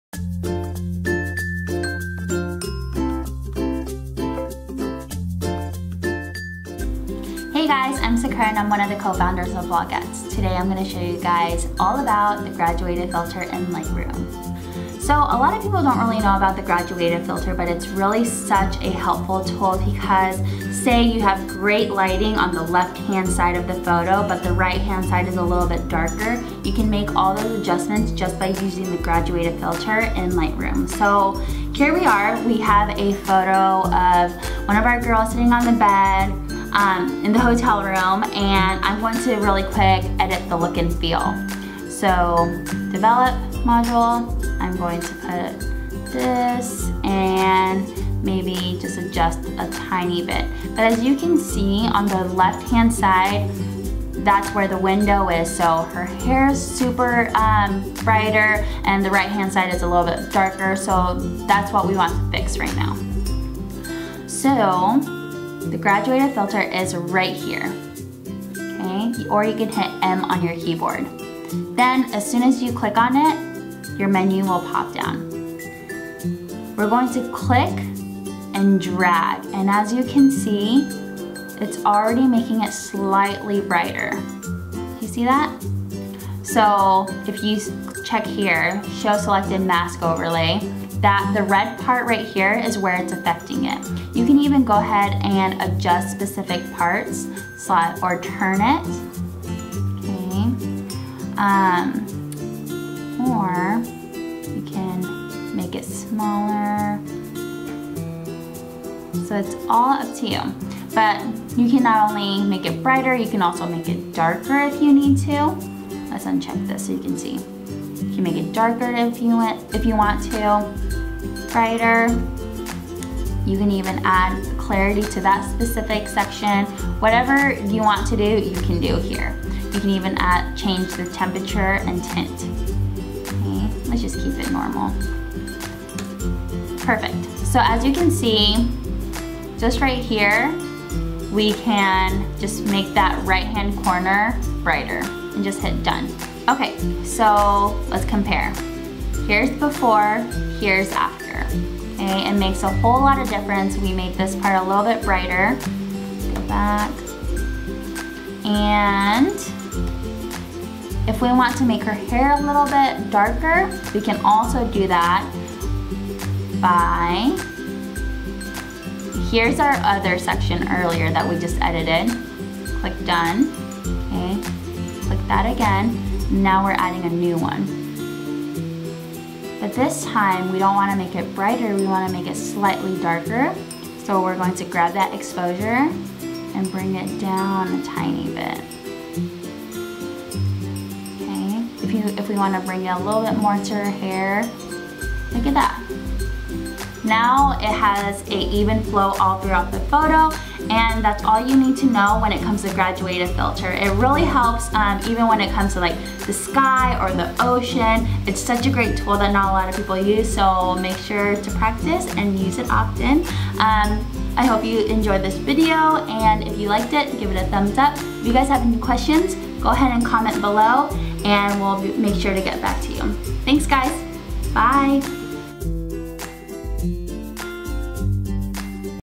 How To: Use the Graduated Filter in Lightroom
One of our co-founders, Sakura shows how to effectively use the graduated filter in Lightroom! It's a quick an easy tutorial that can improve your photos greatly! Lightroom can be a tricky software to attack but that's why we are here. Sakura is very experienced with using Lightroom and she can definitely answer all questions or concerns successfully and genuinely. Have a question that we didn't answer in the video? Make sure to also give a thumbs up so we know to keep making tutorial videos.